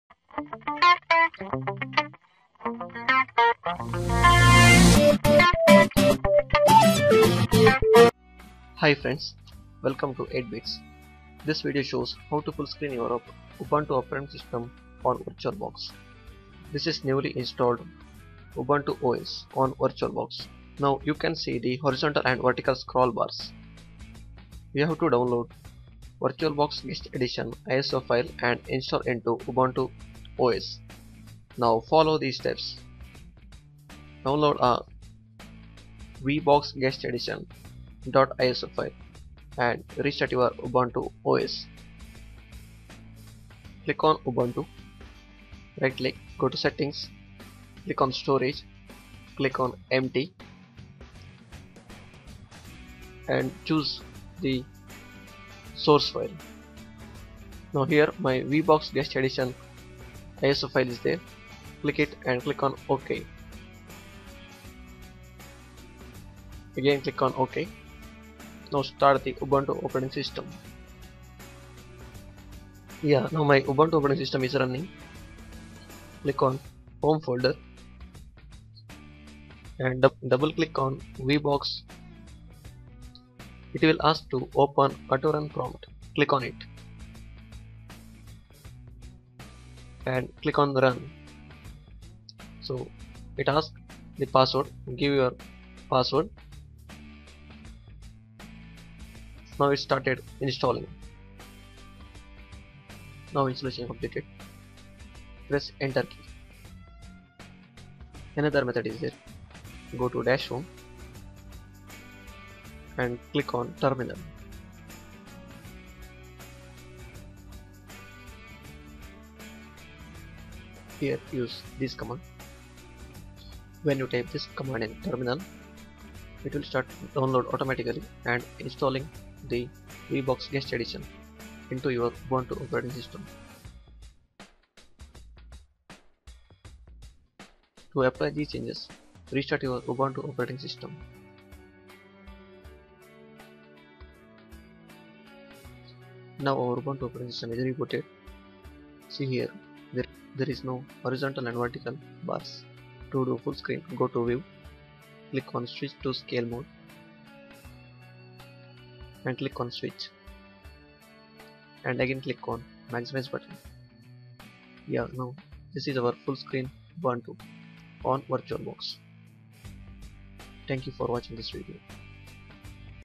How to get Full Screen on VirtualBox [2013]
This video shows 'How to use Full Screen Ubuntu OS on Virtual Box'. This Technic applies to Linux Mint OS also. There are 2 methods, watch the video and learn both methods. This video useful for Linux lovers.